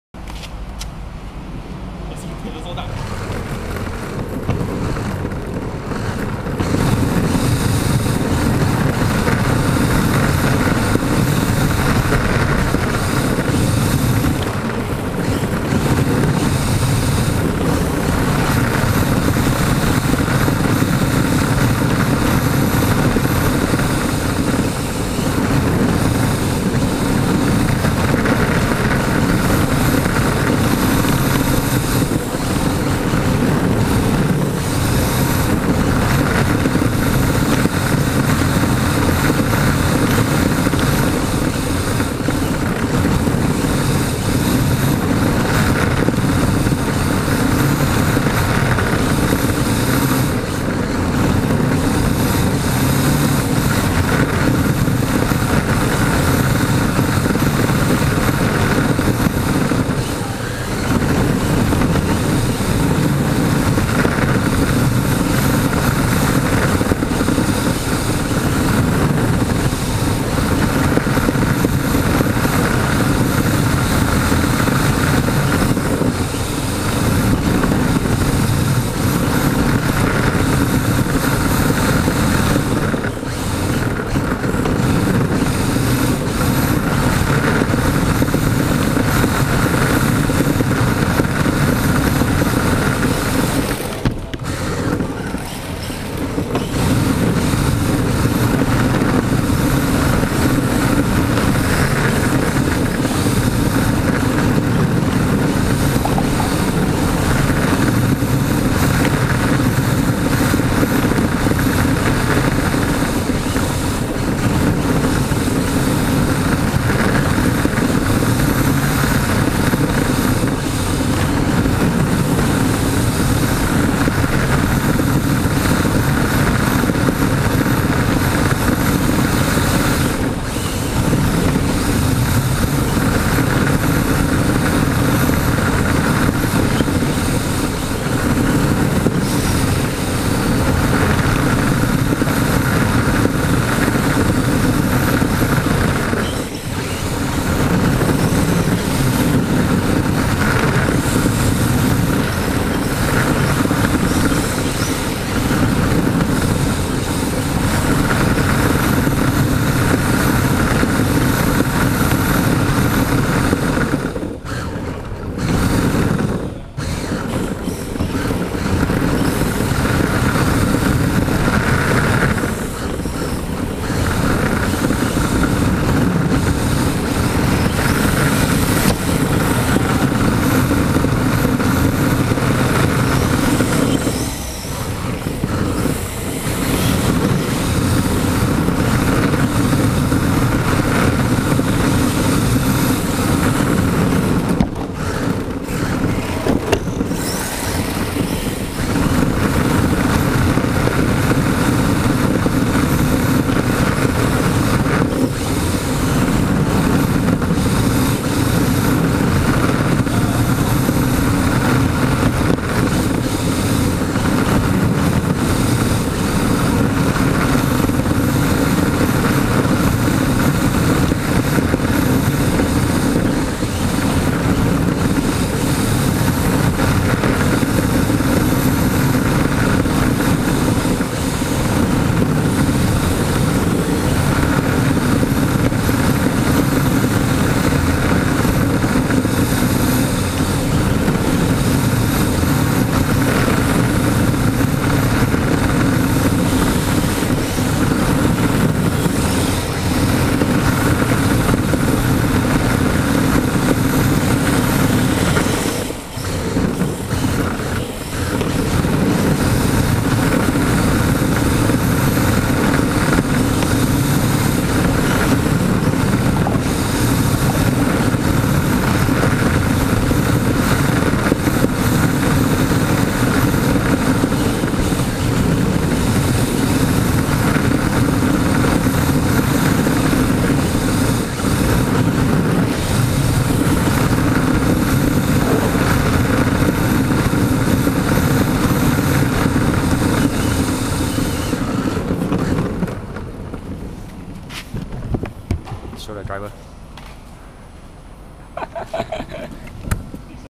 Damien's Yokomo DIB Drift car with GoPro 3 (12th Jan 2014)
Yokomo DIB Full carbon chassis
MarkX Shell

HobbyWing 10.5T BL Sensored Motor
Reventon 140A Sensored ESC
Align 650 Servo

Futaba 4PLS Tx

Driver : Damien Tan
Video : GoPro 3